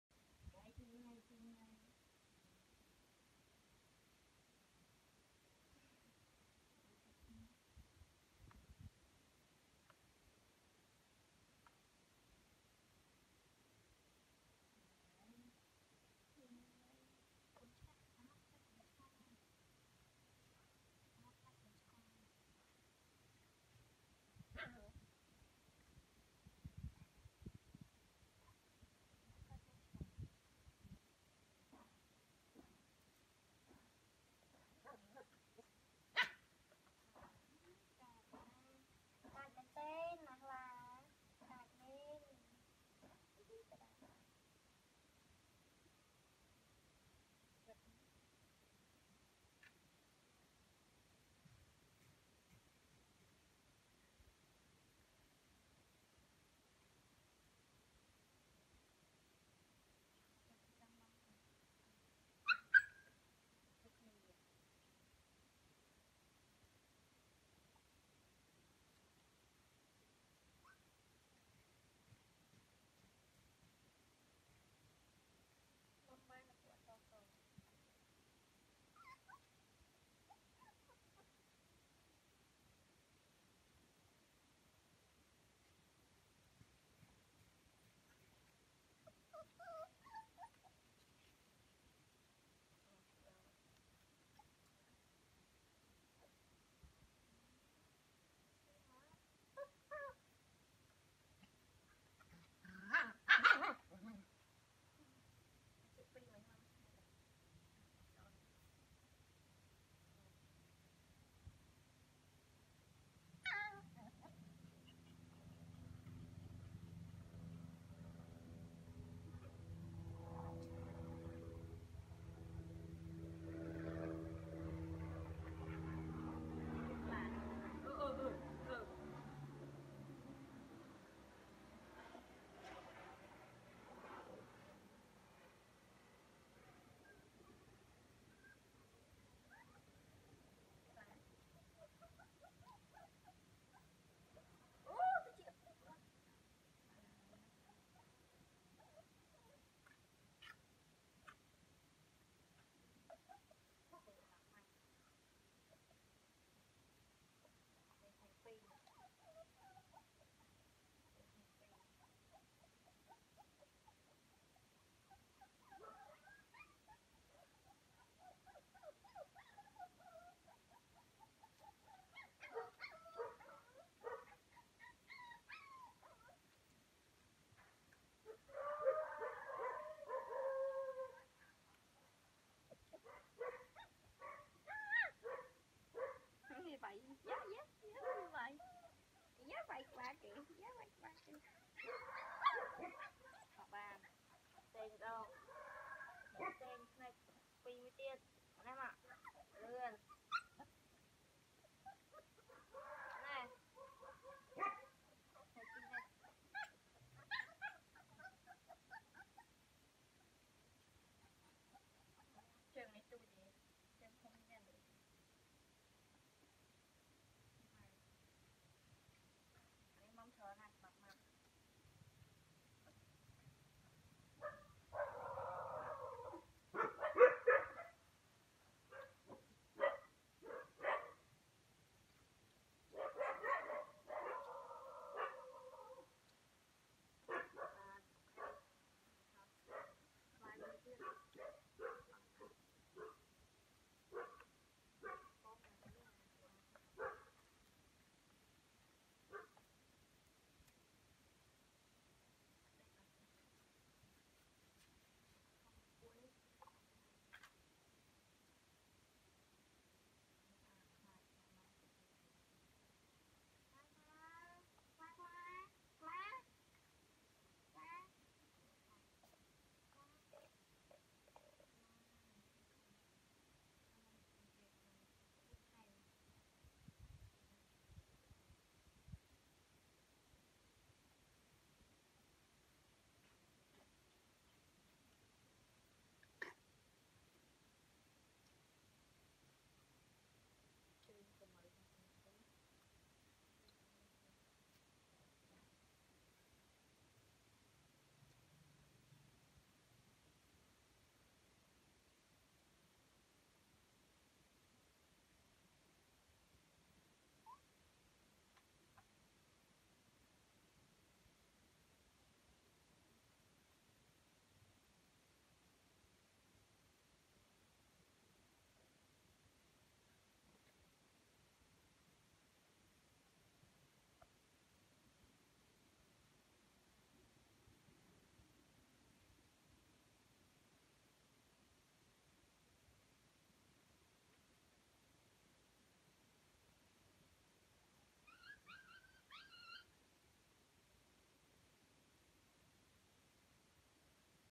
Feeding milk to Starved puppy by using Bottle Feeder
Using Baby Bottle Feeding Milk to Feed The Starving Puppies.
They are so lovely. They became starving since their mother was gone. It is so sad. for now, they have been treated and taking care of a kind-hearted woman who lives there.